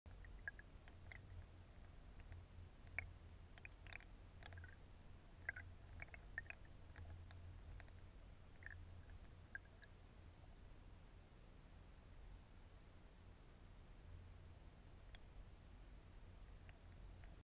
Игра приманки под водой. Es Easy Shiner 125mm от Esfishing. Рыбалка.
Игра приманки под водой на течении. Подводная съёмка.